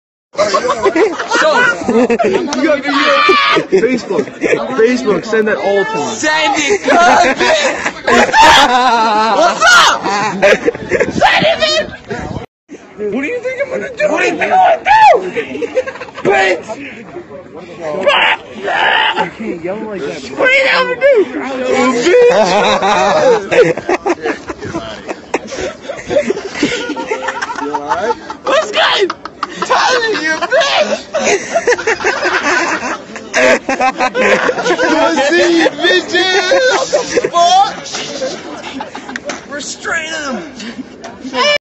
Kid Drunk As Fuck @ Reefer Fest
1 bottle of moonshine + a pint of whiskey = whatever this is
Reefer Fest, Gary IN 12/9/17